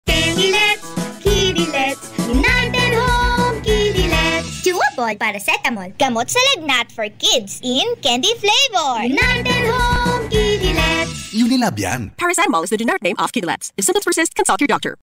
United Home Kiddilets Radio Ad 2022 15s
Hey kids! United Home released their own radio ad for Paracetamol (Kiddilets) for 2022.

DRPS Two 1922: United Home Kiddilets Radio Ad 2022 15s